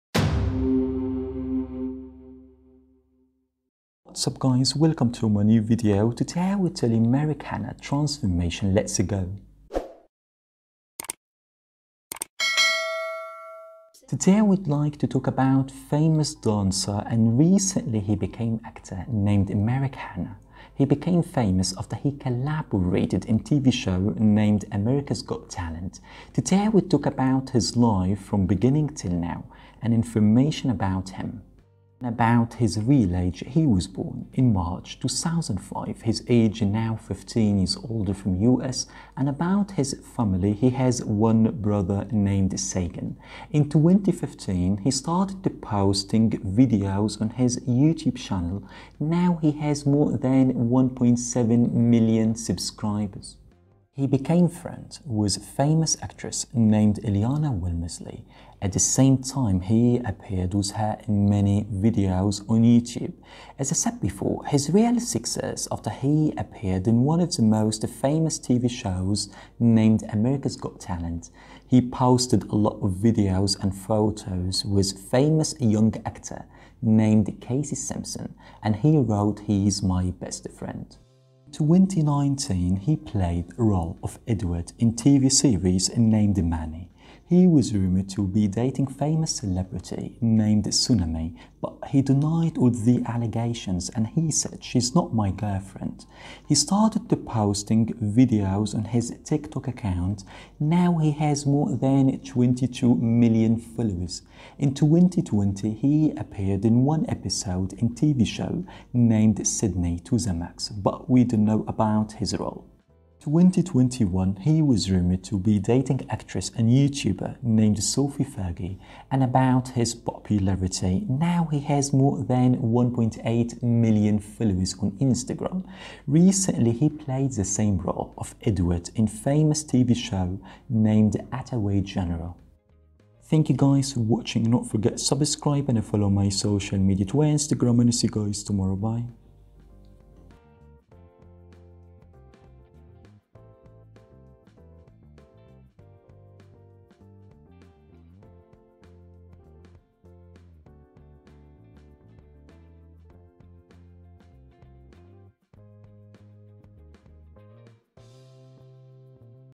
Merrick Hanna - Transformation From 1 to 16 Years Old
Merrick Hanna
Today I'll tell you about Merrick Hanna from beginning until now and more information about him.